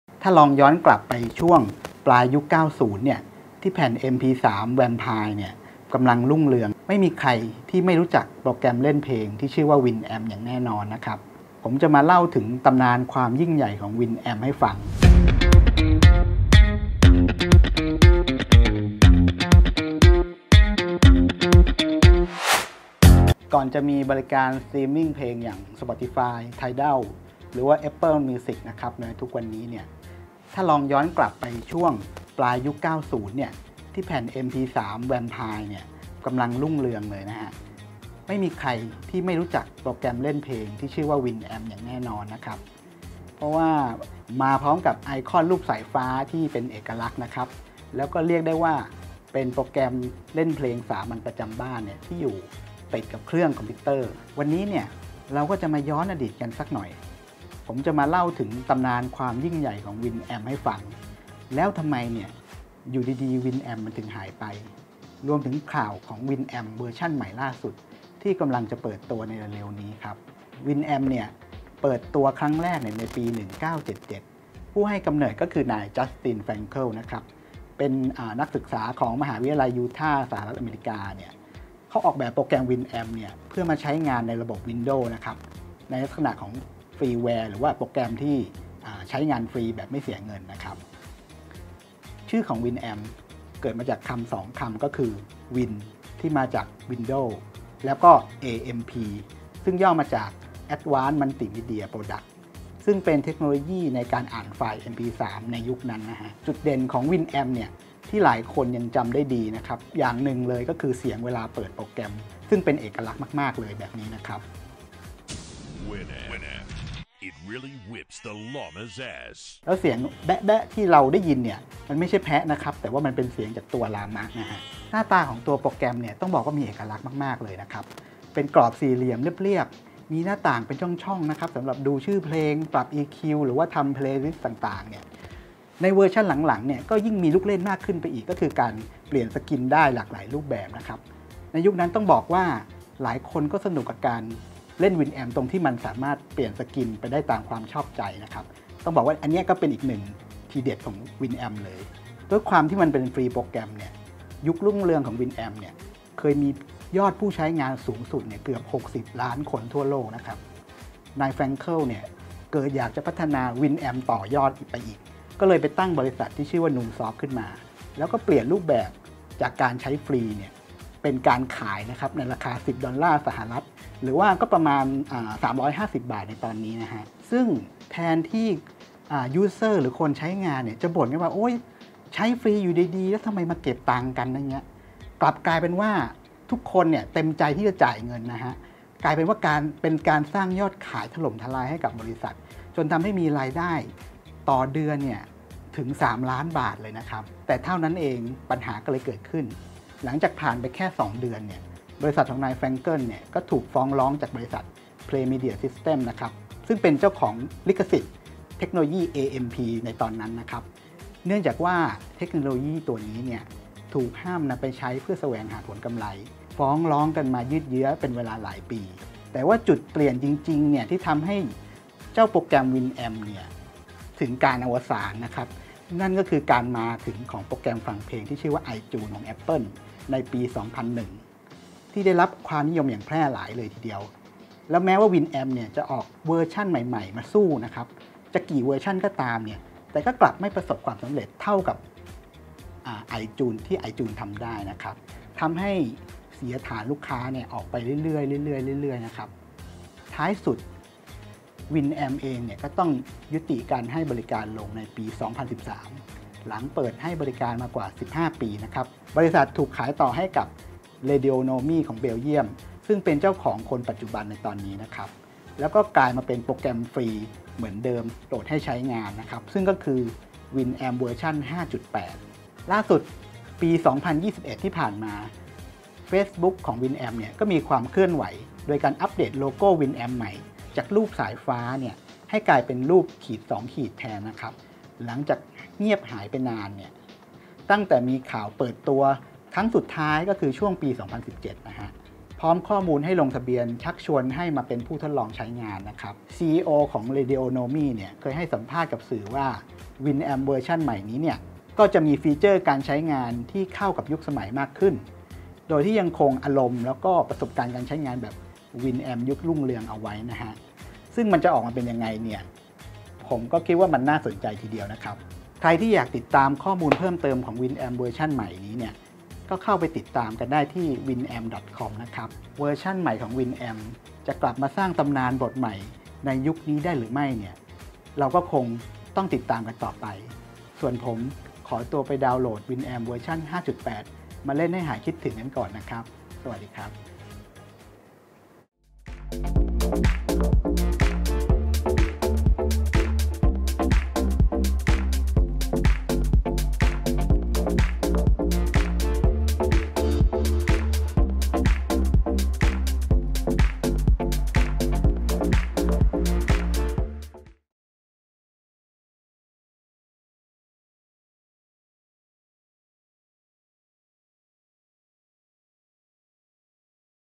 Winamp ยังไม่ตาย? เจาะเวลาหาอดีตกับแอปเล่นเพลงในตำนาน
เรื่องราวของ Winamp แอปฟังเพลงในตำนาน และข่าวของเวอร์ชั่นใหม่ล่าสุดที่กำลังจะเปิดตัวเร็ว ๆ นี้